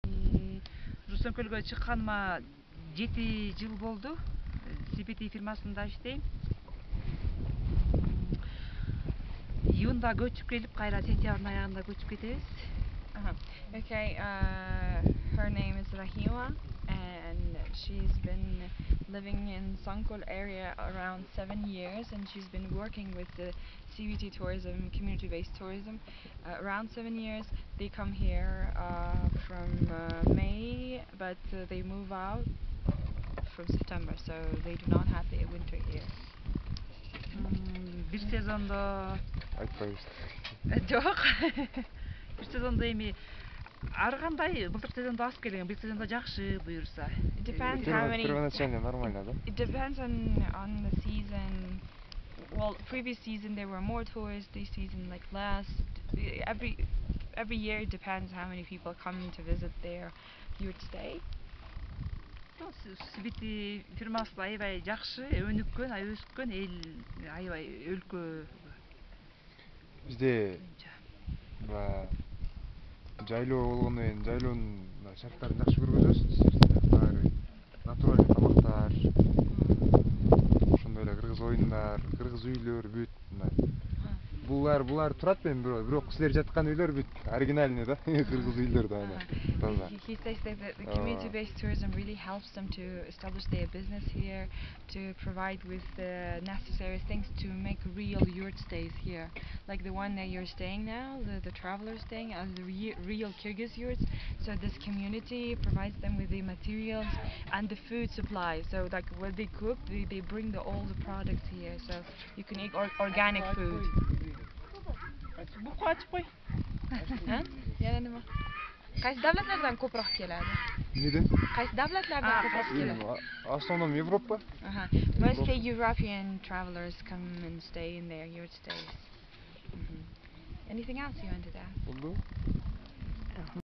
Kyrgyzstan Yurt Host Spotlight #1
During our GAdventure Best of the Stans tour I was able to interview our Kyrgyzstan yurt hosts.